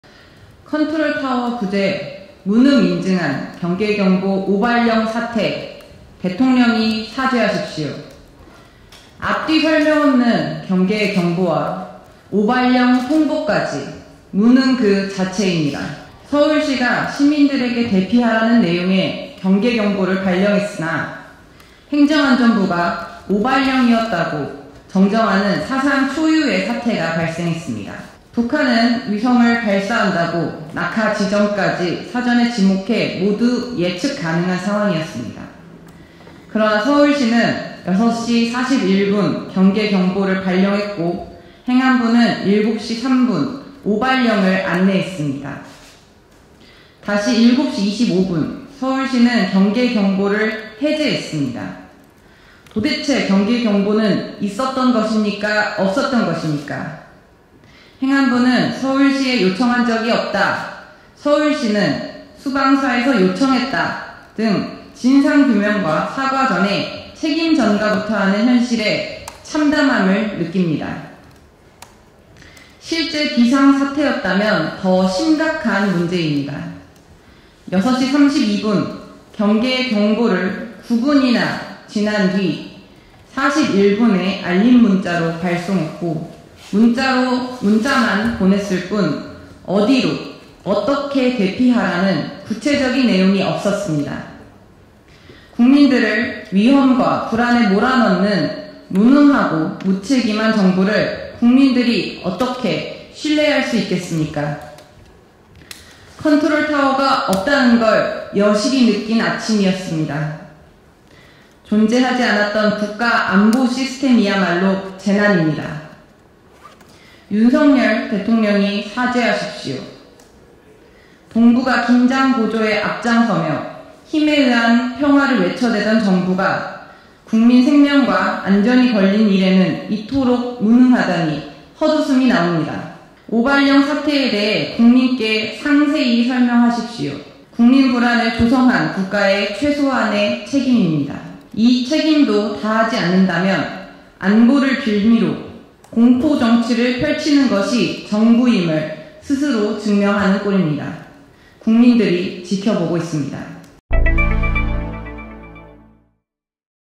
경계경보 오발령 사태. 본질을 꿰뚫는 뼈직구 "컨트롤타워 부재가 재난" (진보당 손솔)
2023년 5월 31일 손솔 진보당 대변인의 국회 브리핑입니다.